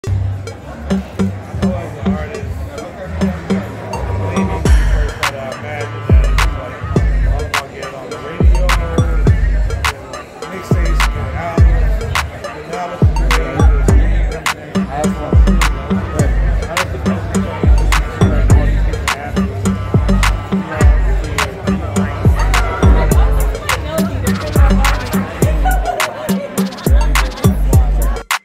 Taja V. Simpson, Ryan Easter & Walter Fauntleroy
Taja V. Simpson, Ryan Easter & Walter Fauntleroy - premiere of 'Fear' / DGA Hollywood, USA

Licensing available for editorial, broadcast TV, all media and personal use worldwide